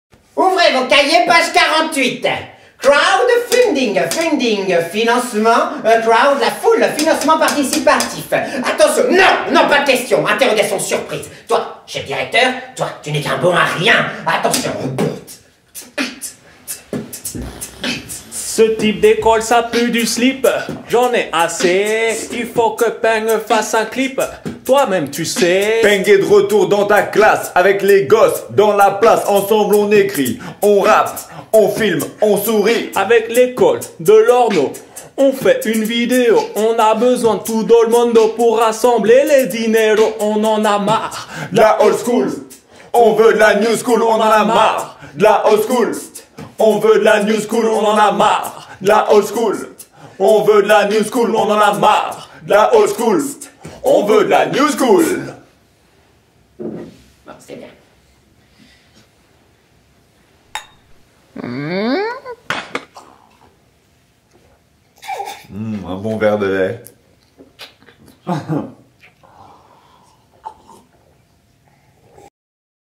PANG + EDO = Rap l'école !
Soutenez notre projet de réalisation d'un vidéoclip destiné à promouvoir l'Ecole Démocratique de l'Orneau et l'enseignement alternatif !